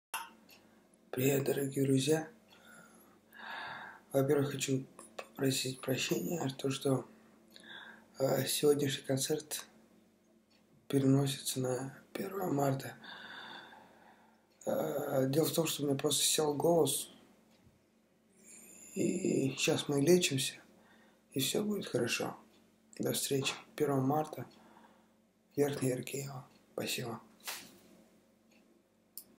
Перенос концерта в Верхнеяркеево с 19 февраля на 1 марта. 2016
ОРГАНИЗАЦИЯ КОНЦЕРТОВ:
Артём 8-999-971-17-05